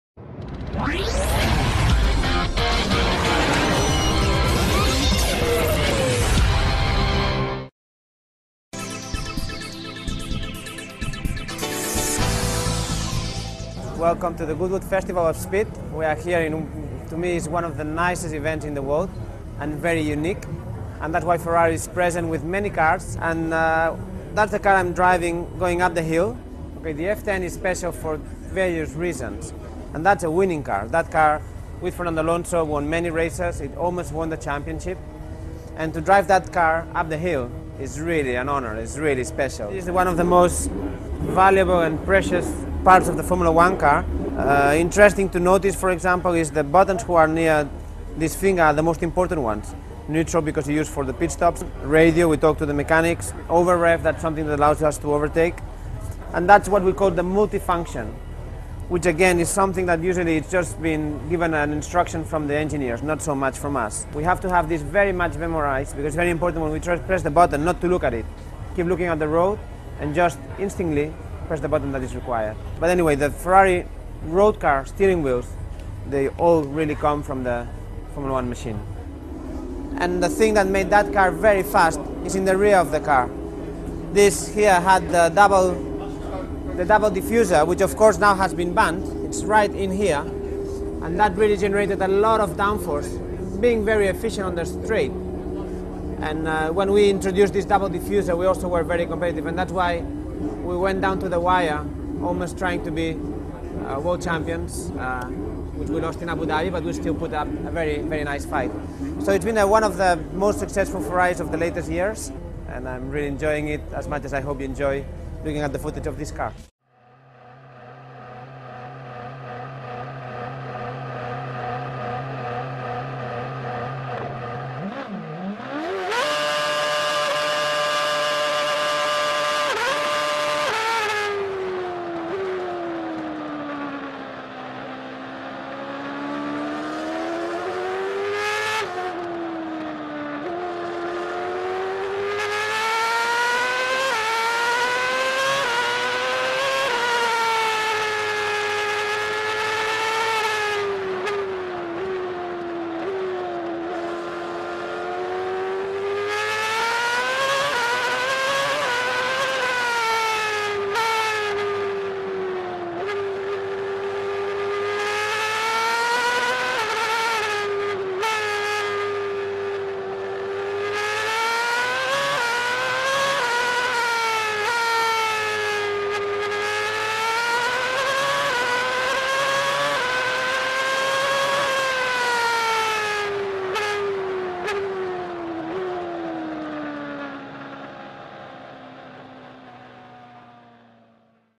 Marc Gene Ferrari F1 en Goodwood - PRMotor TV
Como todos los años, Ferrari asiste al festival de Goodwood con sus pilotos.
En esta ocasion, Marc Gene nos muestra los detalles del Ferrari F10 antes de su salida a pista, con camara on board incluida!
Special thanks to Ferrari Media for the videos showed.